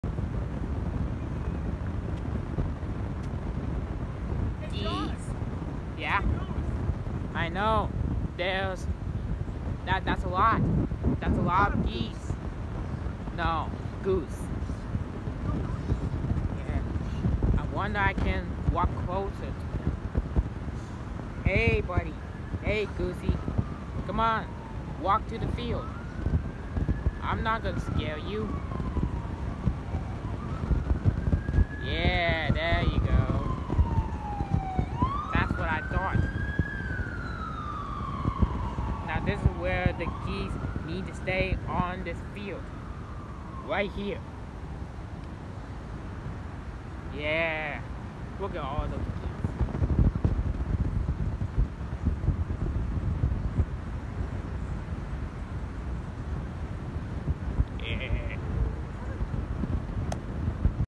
The Geese walks to the field
I was outside walking on the track near in Teaneck High School, and there was a 1 geese (or you should call it goose) stands on the track, then he walking in the field while I tell the geese to to go on the grass.

Question: Have you ever seeing some Greece or goose anywhere in this world?

Note: This video was recorded on September 24, 2024.